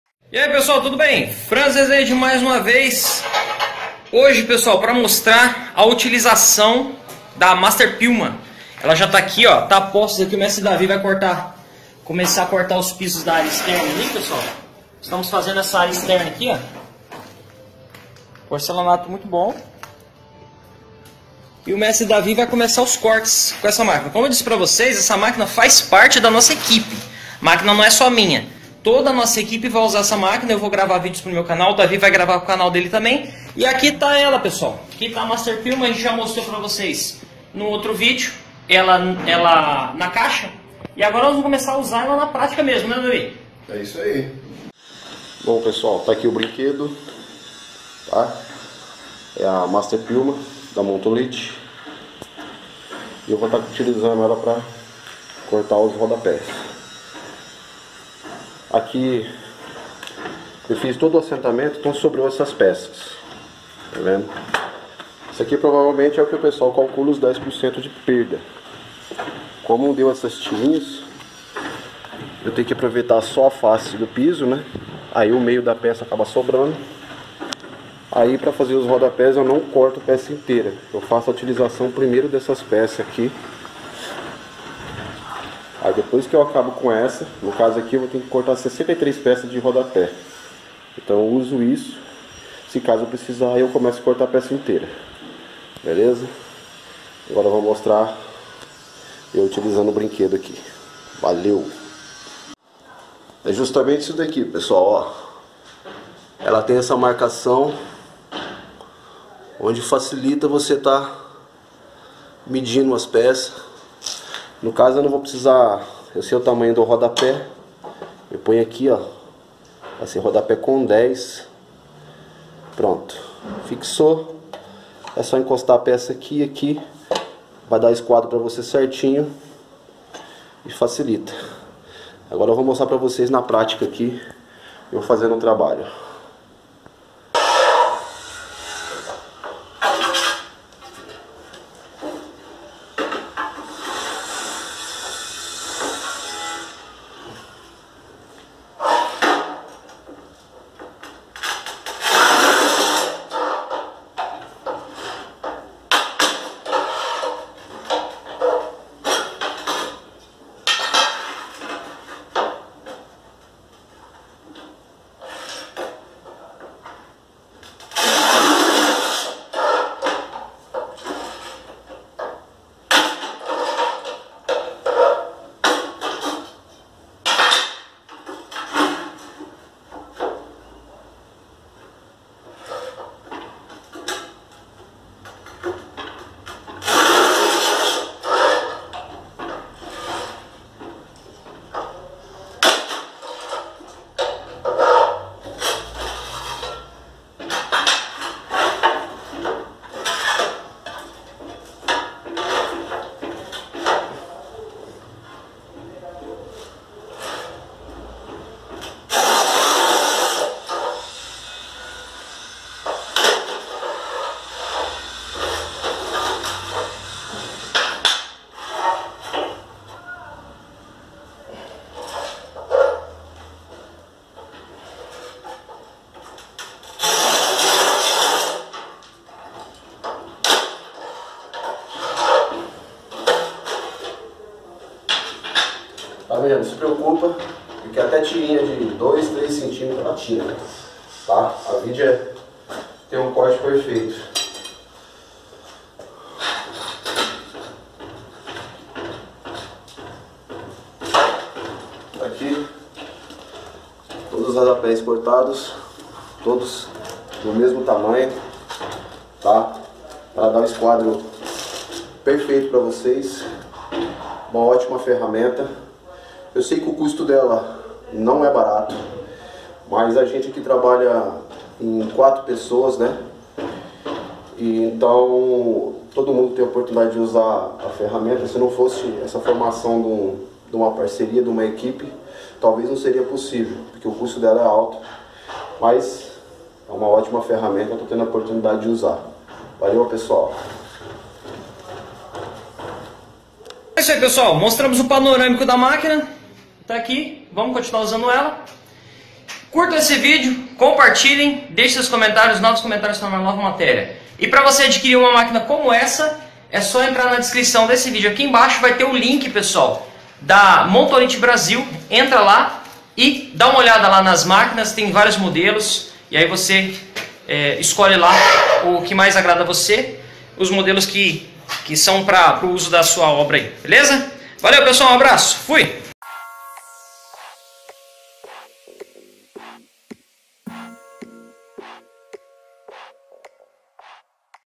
Assentamento de porcelanato com aproveitamento quase total,sem desperdício.
O porcelanato bem assentado e bem paginado tem uma aproveitameto quase total das peças.
Os meios da peças recortadas viram rodapés e a sobra de material para discarte é minima.
Com uma máquina de qualidade como a Master Piuma da Montolit isso fica fácil pois não há disperdicio de peças.